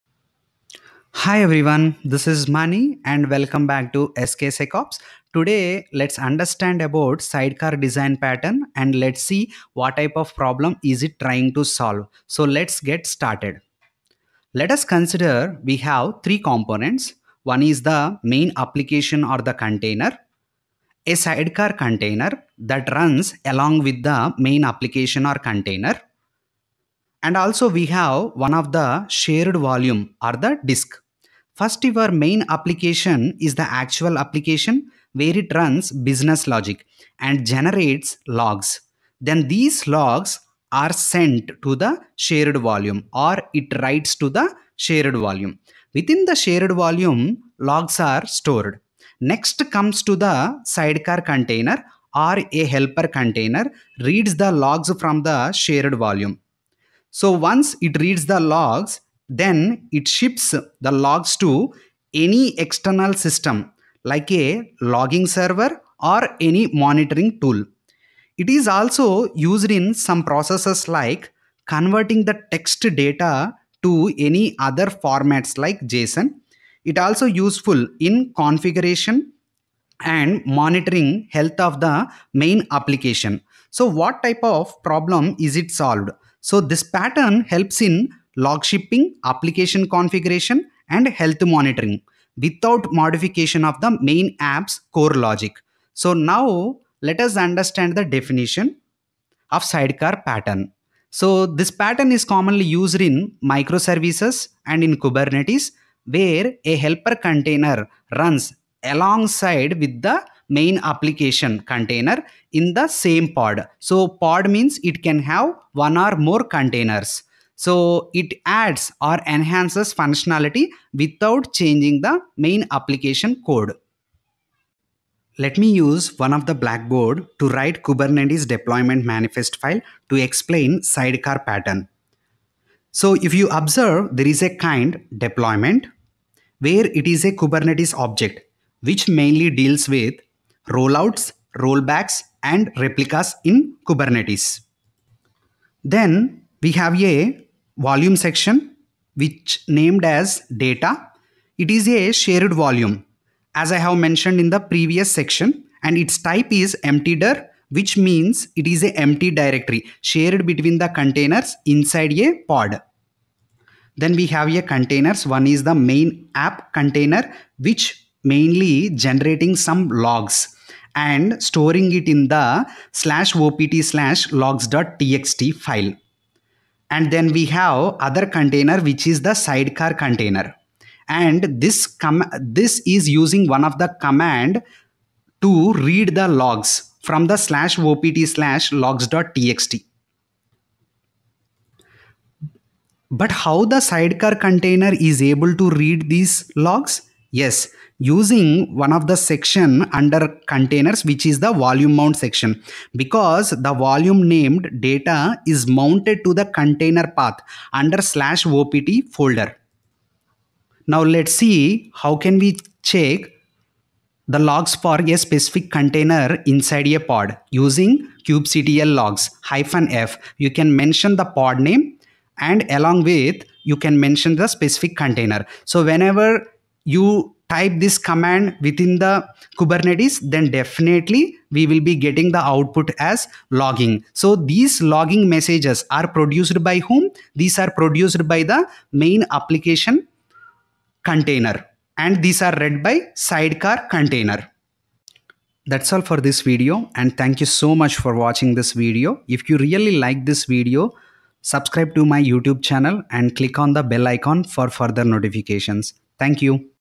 Sidecar Design Pattern in JUST 4 Minutes and Boost Your System Design Skills!
Master the Sidecar Design Pattern in Kubernetes!
In just 4 minutes, learn all about sidecar containers and boost your system design skills with this quick and informative video! Perfect for anyone interested in system design and In this video, we break down the Sidecar Design Pattern, a crucial concept in microservices architecture and Kubernetes deployments. Learn how a helper container can extend the functionality of your main app, especially for logging, monitoring, configuration, and communication with external systems.

🔍 What you'll learn:

What is the Sidecar Pattern?

How the main container and sidecar container communicate

Shared volumes and log collection

Real-world use cases and architecture visualisation

Whether you're a DevOps engineer, cloud architect, or Kubernetes enthusiast, this concept is a must-know to build modern, observable, and maintainable apps!